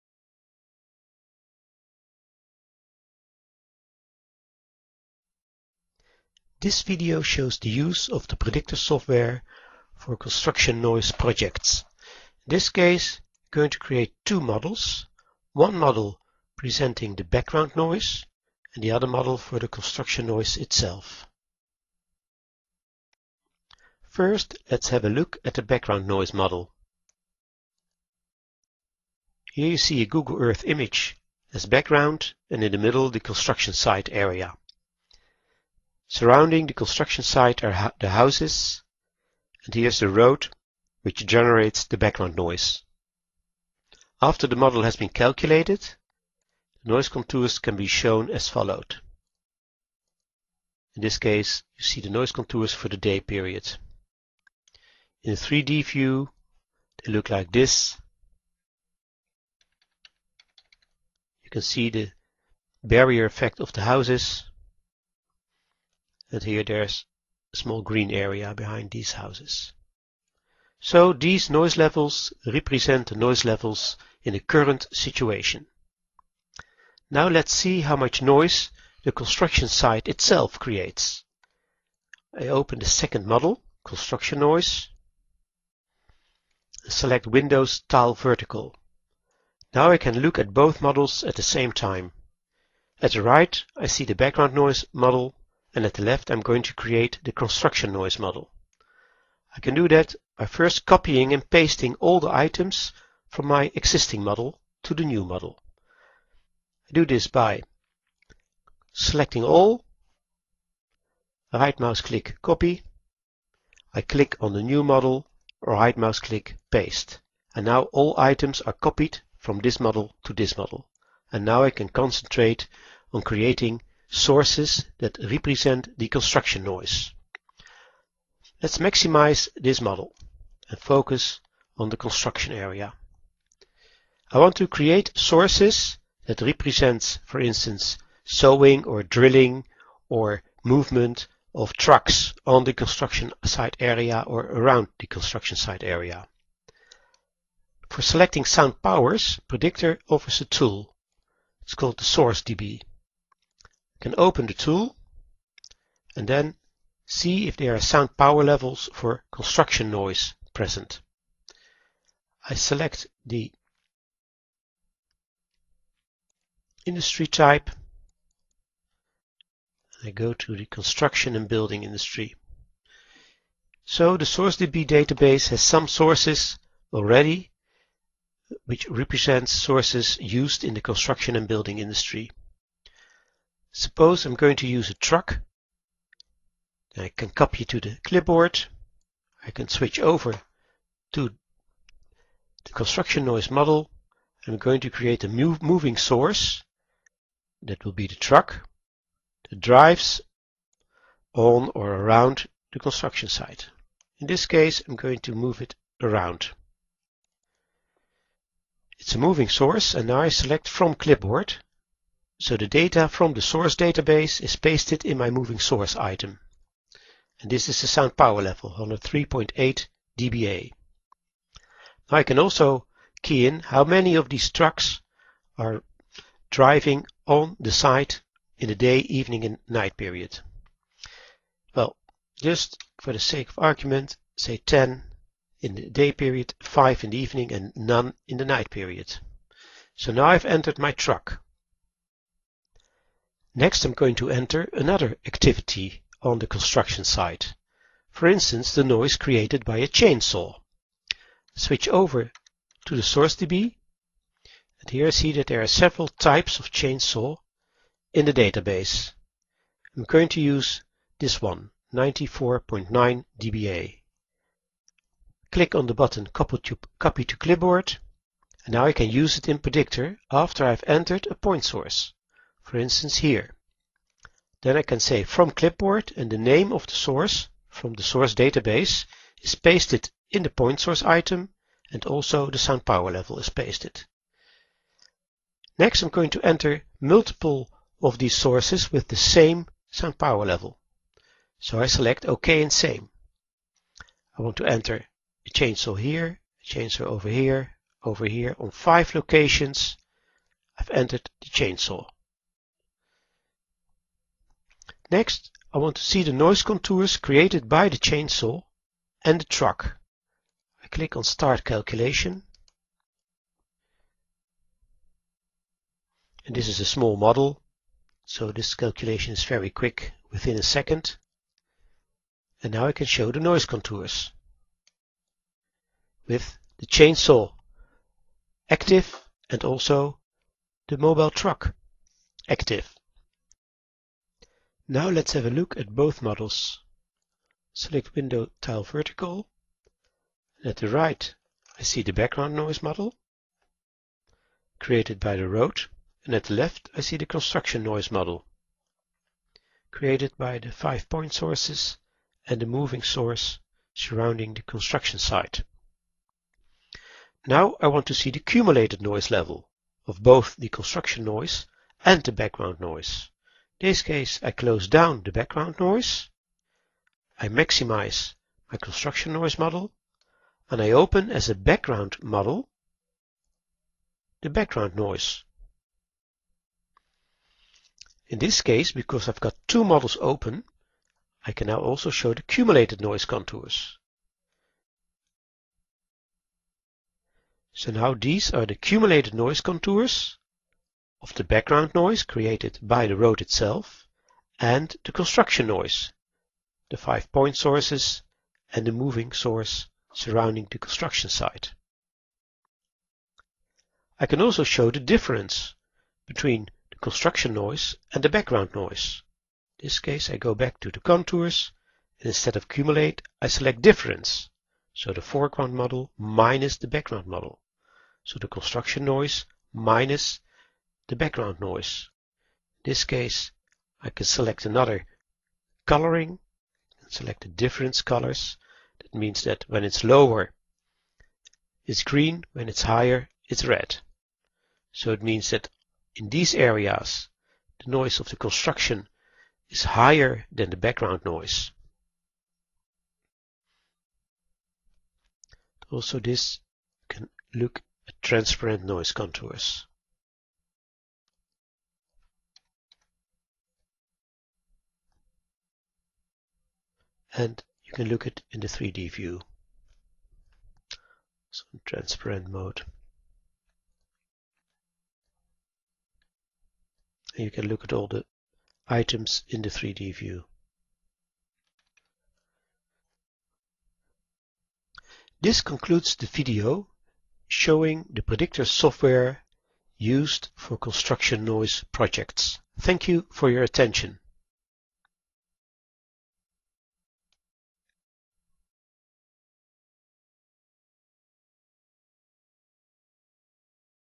Brüel & Kjær Predictor - Calculating construction noise
Uso de Predictor 7810 para modelos de ruido industrial.
Predictor tiene una curva de aprendizaje rápida, lo que le permite trabajar de una manera eficiente, incluso para usuarios poco frecuentes. El modelado es fácil con su intuitivo punto de vista multi-modelo y su única e ilimitada funcionalidad hacer/deshacer. Ser poderoso e intuitivo con macros para modelar los cambios automáticos, puede ayudarnos en la vida real de una forma rápida, fácil y precisa, incluso en situaciones complejas.